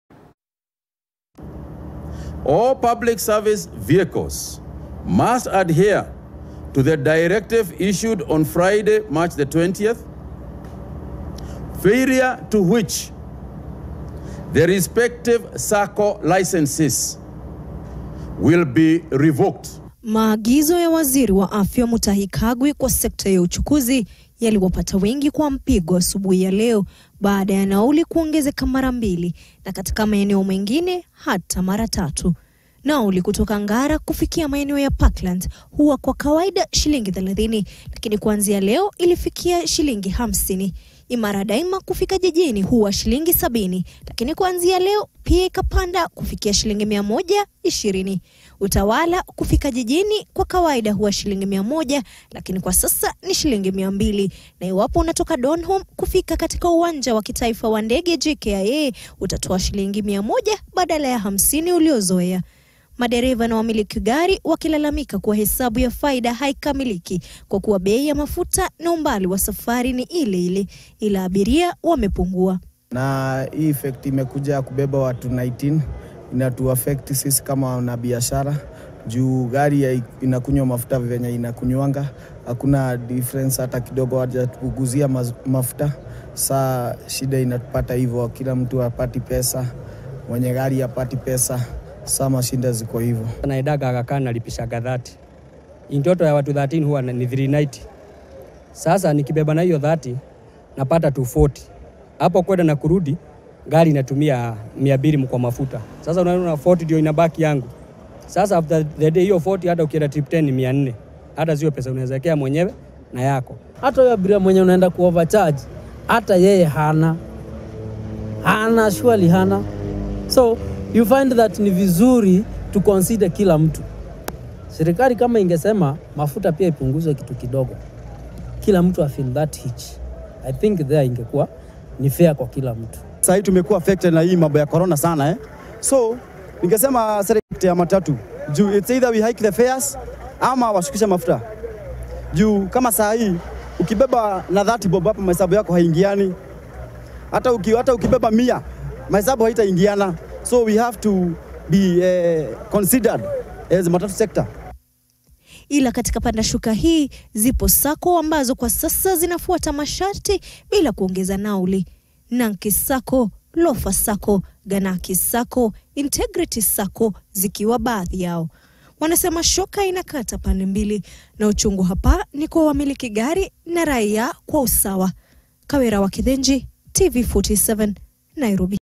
Licenses for matatus that do not comply to be revoked.
CS health reiterates that PSVs that will not follow the new guidelines will have their licenses revoked.